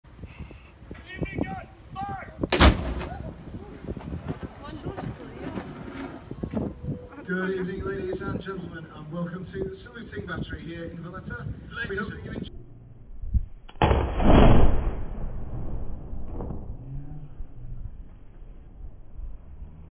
Valletta - Saluting Battery (4K)
The Saluting Battery is an artillery battery in Valletta, Malta. It was constructed in the 16th century by the Order of Saint John, on or near the site of an Ottoman battery from the Great Siege of Malta.